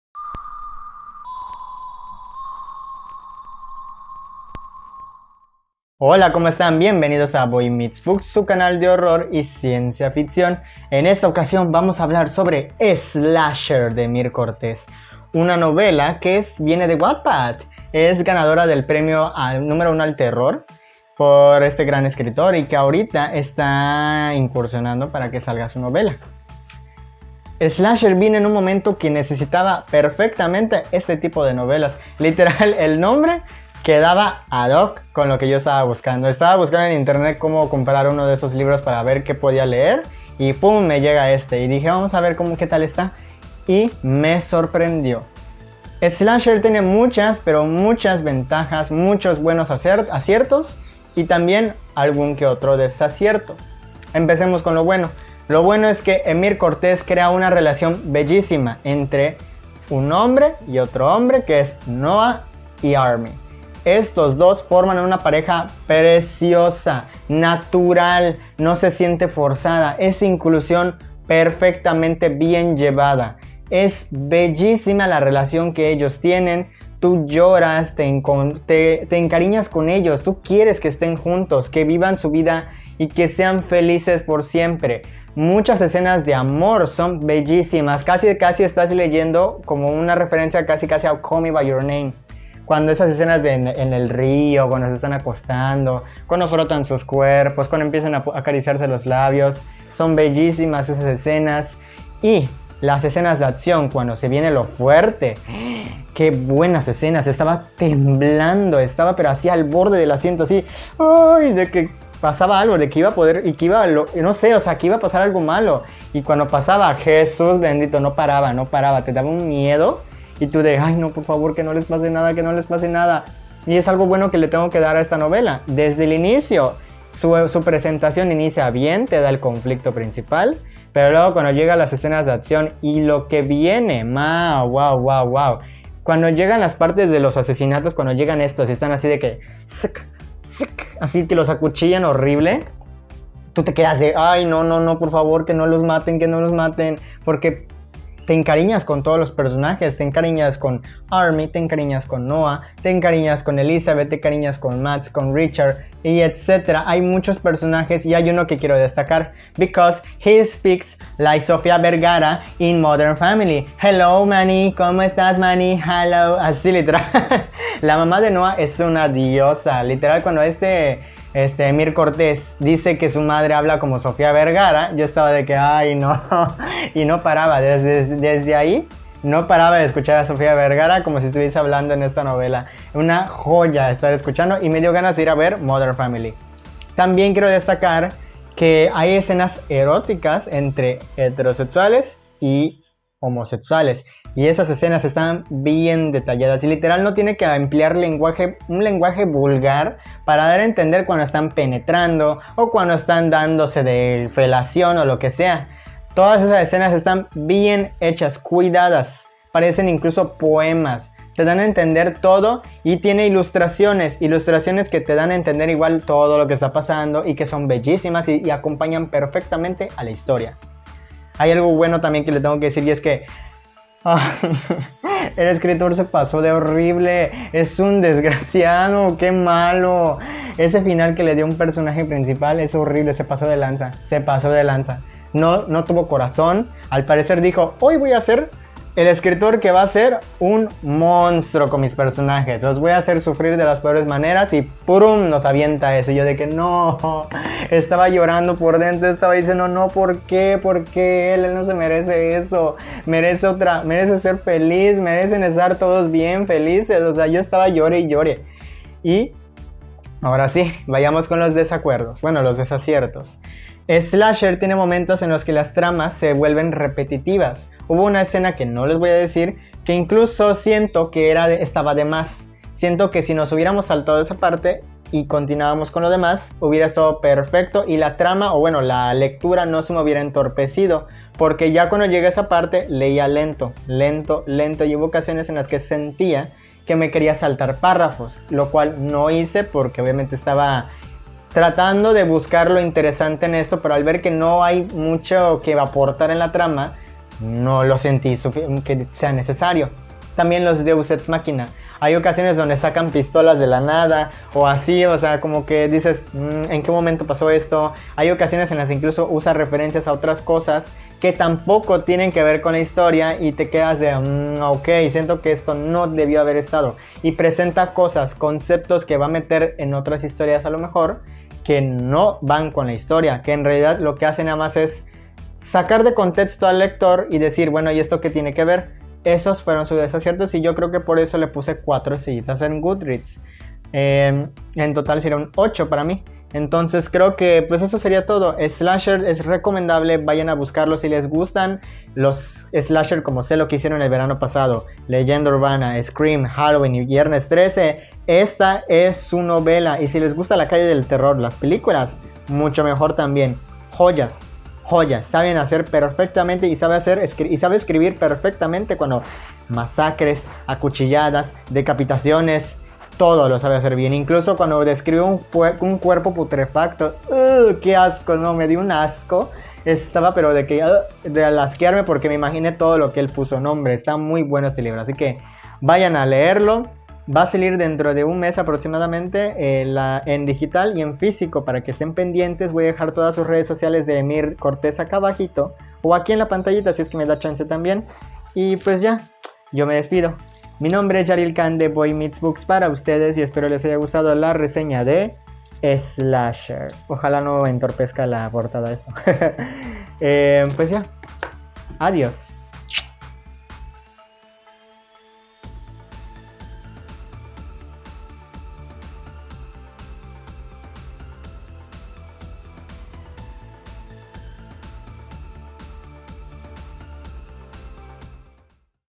SLASHER DE EMIR CORTEZ - RESEÑA
En este video descubriremos todo lo que ofrece la novela del escritor Emir Cortez y sus homenajes a los slasher de los años 80 y 90.